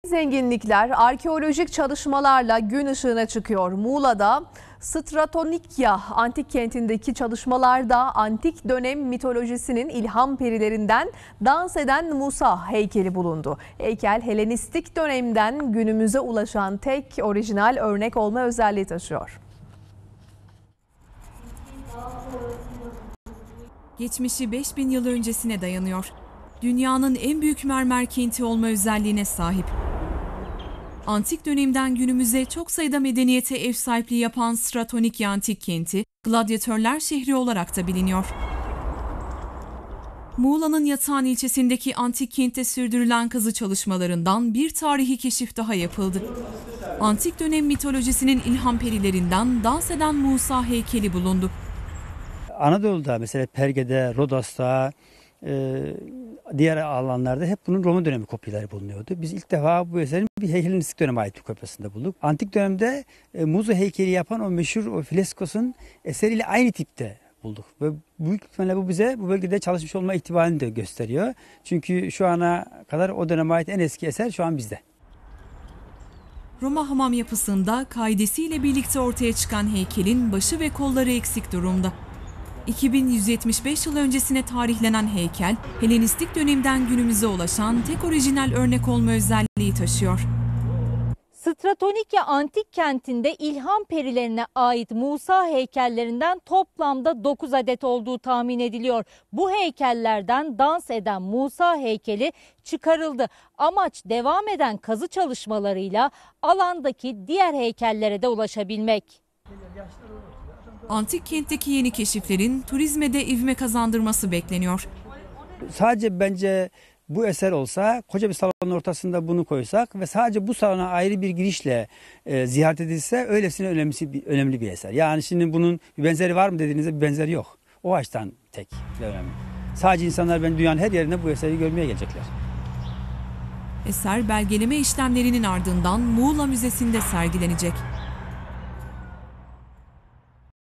GLADYATÖRLER ŞEHRİ STRATONİKEİA'DA YENİ KEŞİF: "DANS EDEN MUSA" HEYKELİ BULUNDU
Tarihi zenginlikler, arkeolojik çalışmalarla gün ışığına çıkıyor.
Muğla'da Stratonikeia antik kentindeki çalışmalarda, antik dönem mitolojisinin ilham perilerinden "Dans Eden Musa" heykeli bulundu.
Heykel, Helenistik dönemden günümüze ulaşan tek orijinal örnek olma özelliği taşıyor.